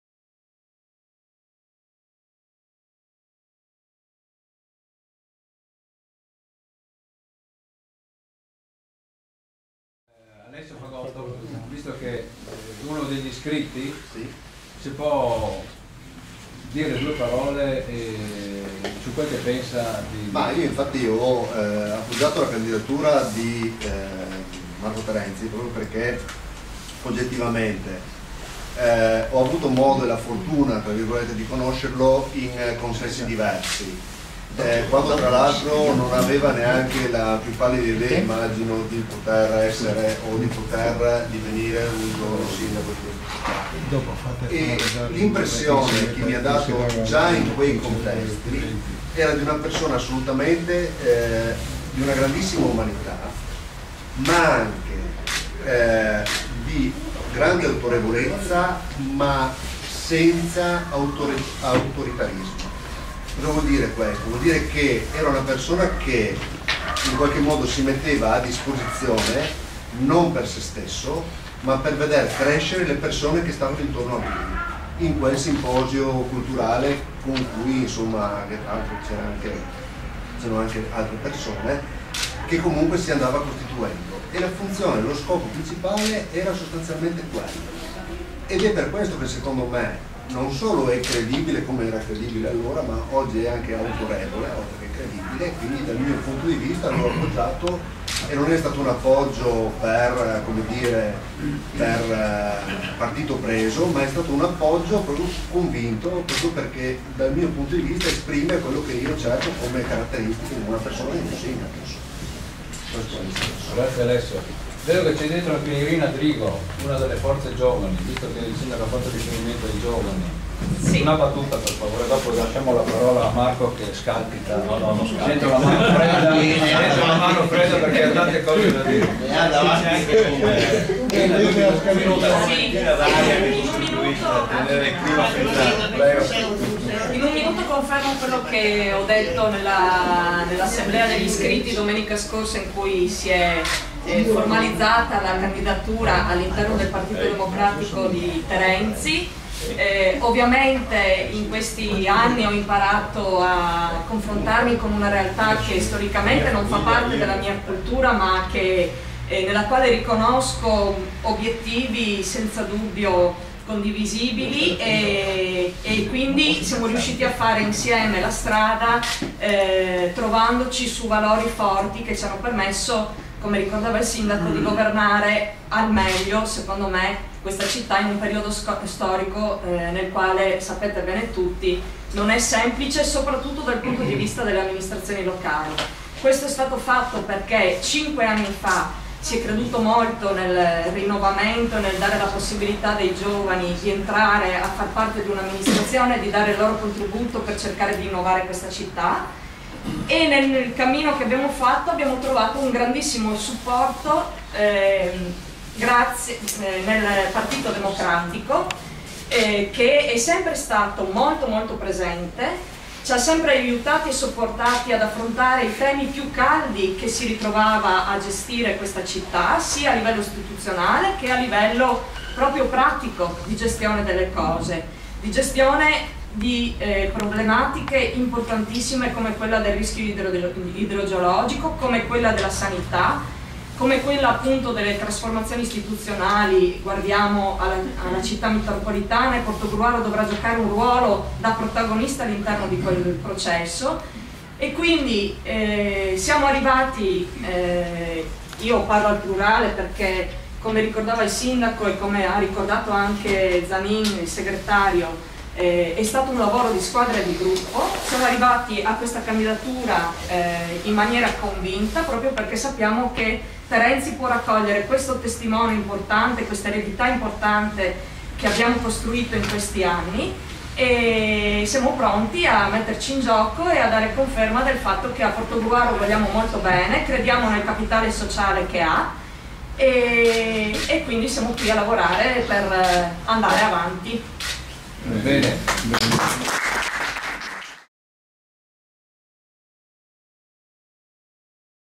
PD: CANDIDATURA TERENZI - COMMENTO FAGOTTO E DRIGO
Alessio Fagotto e Irina Drigo ci dicono perchè secondo loro Marco Terenzi è l'uomo giusto per succedere al sindaco Antonio Bertoncello alla conduzione della città di Portogruaro